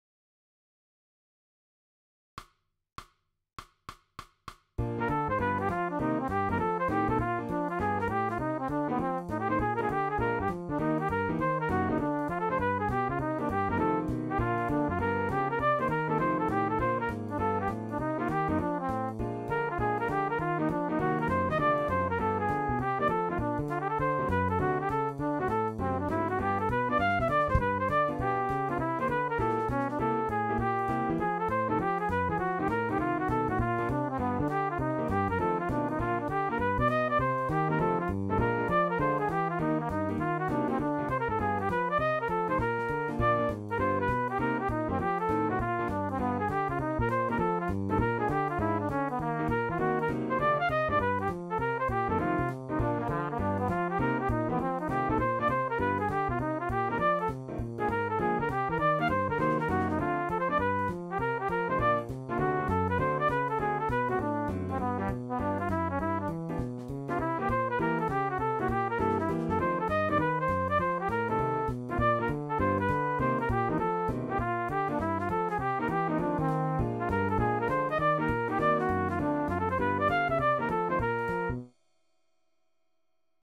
2 Chorus solo on Rhythm Changes, Notation for Bb Instrument
Solo constructed and played using Impro-Visor on Bb Rhythm Changes, notated for a Bb instrument